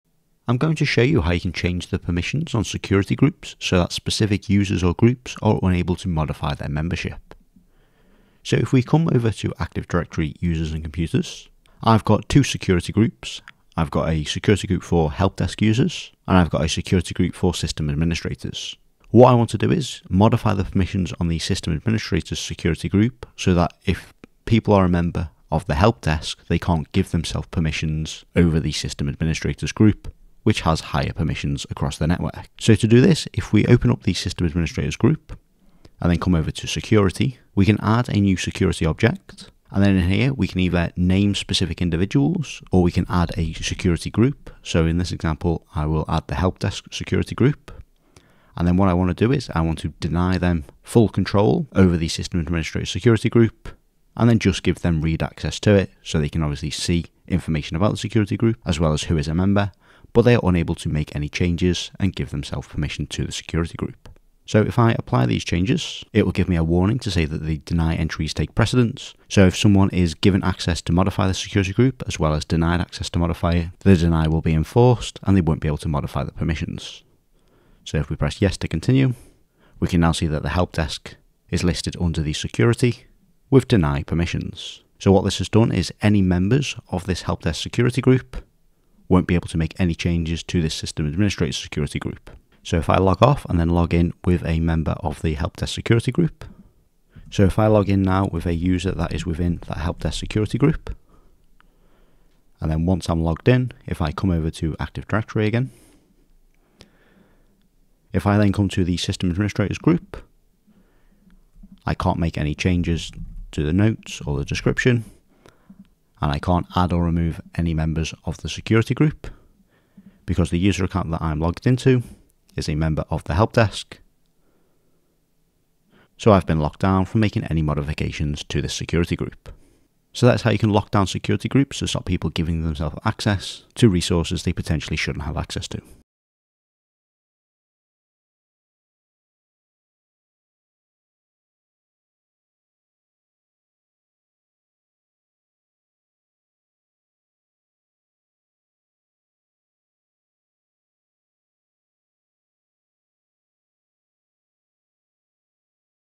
How to restrict Active Directory security groups and block membership modifications
Learn how to setup deny permissions on security groups so that specific users or groups are unable to make modifications to the group membership. In this example, I show you how to amend the security permissions of Active Directory security groups to block specific members or members of security groups from making changes to the initial security group.